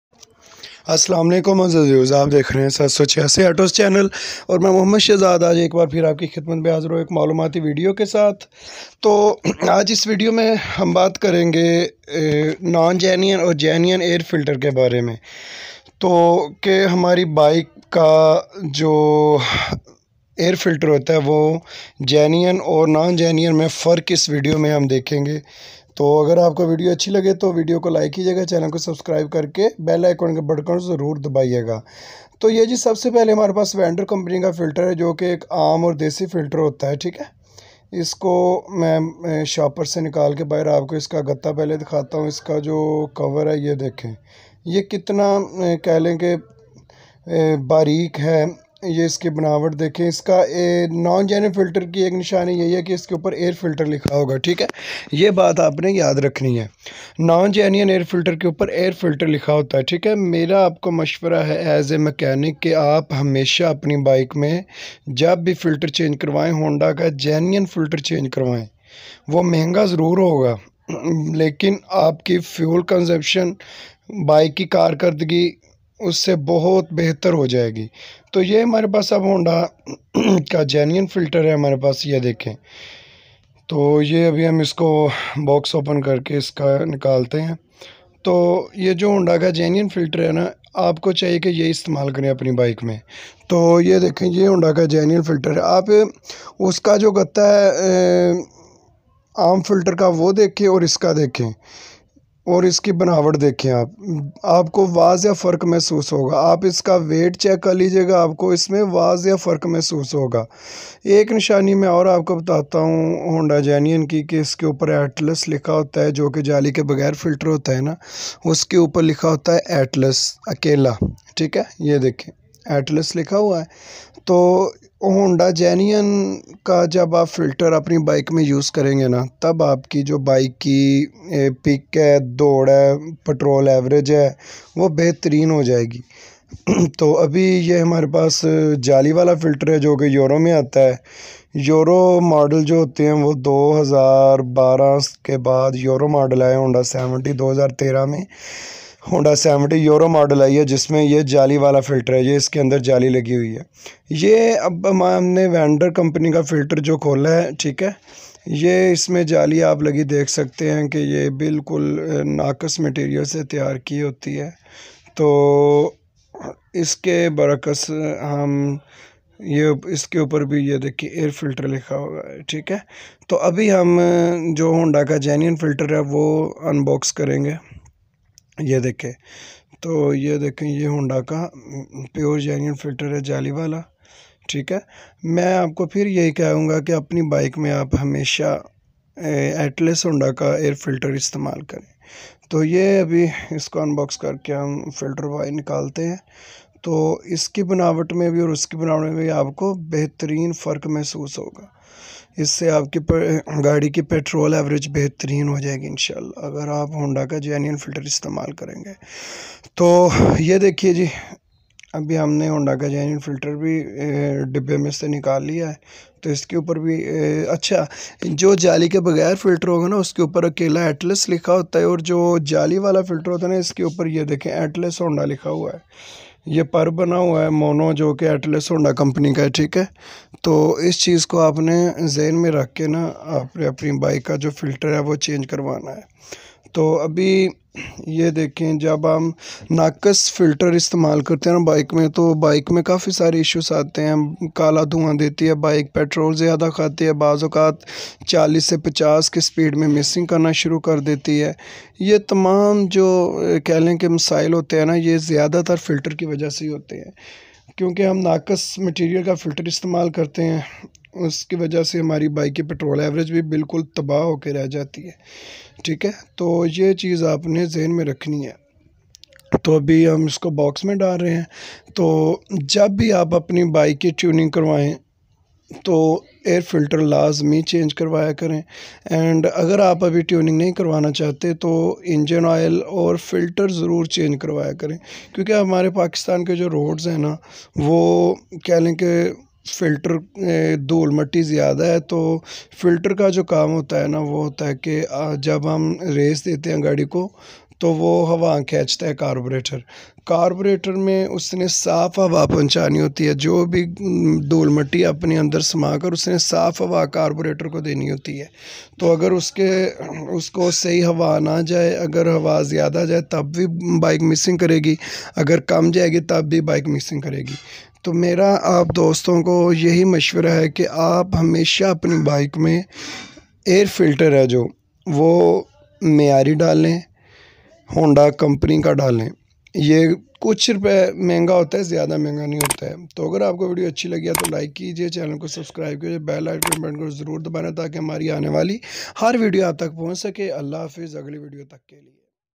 Honda Genuine Air Filter Vs Non Genuine Filter
In this video we will discuss about  waht is differens between genuine and non genuine air filter of motorcycle..
Also known about what is benefits of genuine air filter and what are problems of non genuine filter.